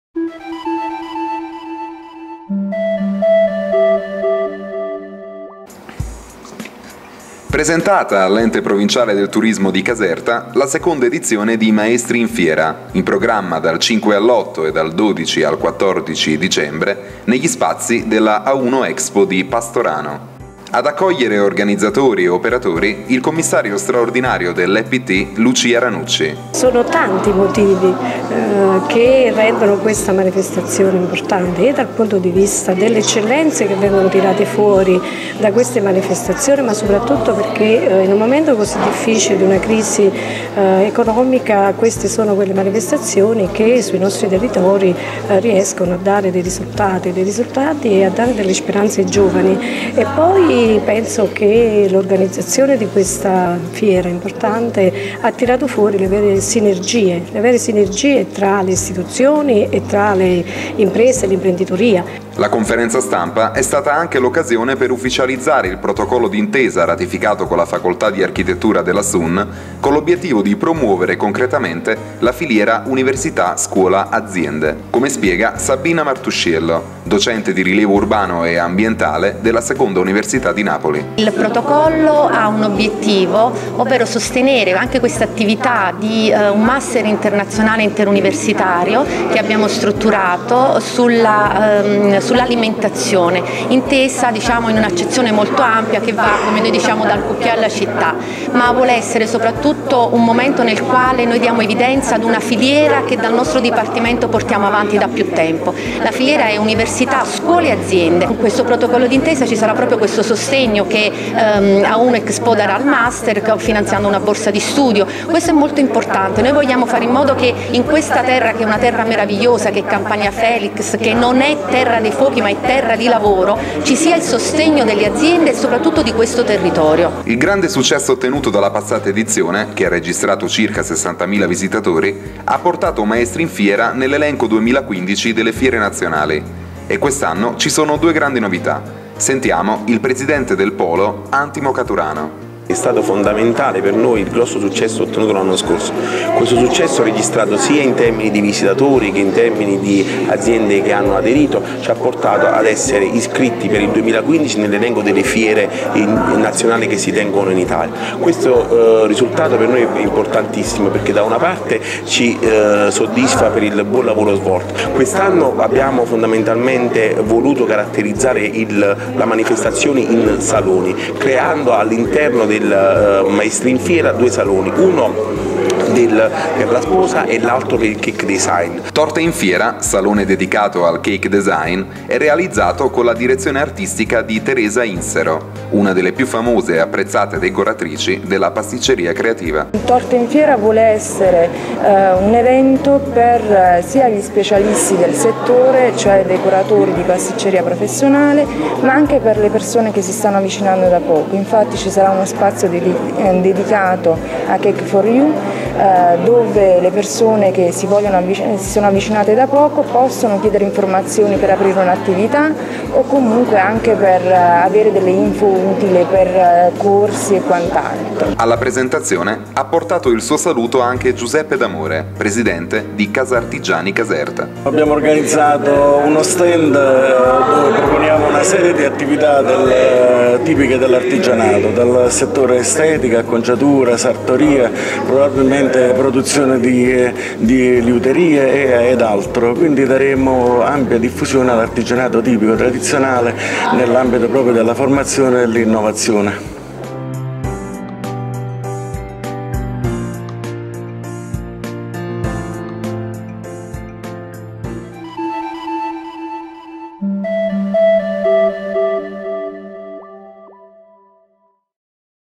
Caserta - Sposi e cake design a ''Maestri in Fiera 2014'' (27.11.14)
- Caserta. Presentata, all’Ente provinciale del Turismo di Caserta, la seconda edizione di “Maestri in Fiera”, in programma dal 5 all’8 e dal 12 al 14 dicembre negli spazi A1 Expo di Pastorano, all’uscita di Capua dell’autostrada A1.

Il commissario straordinario dell’Ept, Lucia Ranucci, ha accolto organizzatori e operatori del settore, accorsi per conoscere le novità che questa seconda edizione porterà con sé.

“È straordinario il modo in cui un’area che prima versava in condizioni di totale degrado sia diventata un fiorente centro di manifestazioni fieristiche”, ha esordito Ranucci, riferendosi al polo A1 Expo. Ed ha poi aggiunto: “Al presidente del polo, Antimo Caturano, va riconosciuto il grande merito di aver saputo creare sinergia sul territorio, dando una spinta alla voglia di recuperare l’orgoglio del nostro essere casertani”.

Dello stesso avviso anche Sabina Martusciello, professore di Rilievo Urbano e Ambientale della Seconda Università di Napoli: “Il nostro obiettivo non è soltanto insegnare ai ragazzi, ma ridare loro la speranza, l’amore per questa terra che non è Terra dei fuochi, ma è Terra di Lavoro. E solo grazie a imprenditori “illuminati” come Antimo Caturano, tutto questo è possibile”.

La conferenza stampa è stata anche l’occasione per ufficializzare il protocollo d’intesa che A1 Expo ha ratificato con la Facoltà di Architettura della Sun con l’obiettivo di promuovere concretamente la filiera Università- scuola-aziende. L’intesa si realizza attraverso il Progetto di Ricerca Applicata “Diaeta Mediterranea: Landesign/ALI-ment-azione”, che ha tra le sue finalità il recupero degli spazi esterni delle Istituzioni scolastiche per divulgare valori, cura, rispetto, partendo dalla terra, facendo vivere la progettazione come momento di partecipazione collettiva. E proprio l’artigianato è progettazione, è creatività, è passione.

“Gli artigiani, con il loro sapere e il loro saper fare, sono il collegamento tra passato e presente, l’identità di un territorio» ha affermato Caturano. «Questa consapevolezza – ha aggiunto il presidente del polo fieristico – unita al grande entusiasmo che abbiamo riscontrato tra gli operatori del settore, ci ha spinto a dare vita alla seconda edizione di Maestri in Fiera, che già lo scorso anno ha avuto un grandissimo successo con i suoi circa 60mila visitatori. Proprio questo grande successo ottenuto lo scorso anno, ha determinato, da un lato, l’iscrizione di Maestri in Fiera nell’elenco 2015 delle fiere nazionali che si tengono in Italia; dall’altro, un incremento del 10% degli espositori provenienti da diverse regioni italiane”.

La novità principale di questa nuova edizione è “Nozze in Fiera – Salone Mediterraneo della Sposa”, un evento nell’evento rivolto alle coppie che si apprestano a compiere ‘il grande passo’. “Nozze in Fiera” riunirà tutti gli operatori della filiera artigianale del matrimonio, ponendosi come trait d’union tra i futuri sposi e le eccellenze della produzione artigianale che la Campania esprime nel settore.

L’altra grande novità è “Torte in Fiera”, il salone dedicato al cake design realizzato con la direzione artistica di Teresa Insero, una delle più famose decoratrici conosciutissima e apprezzata negli ambienti della pasticceria creativa. “Torte in Fiera si avvale di ben 20 sponsor di altissimo livello, tra cui Pavoni e Lo Conte. – ha detto – Avremo con noi ospiti d’eccezione come Antonio Crispino (concorrente del programma di Real Time ‘Bake Off Italia’, ndr) ma anche dimostrazioni e la grande prova ‘live’, che vedrà sei squadre in gara per creare la torta migliore e vincere i premi in palio”. (27.11.14)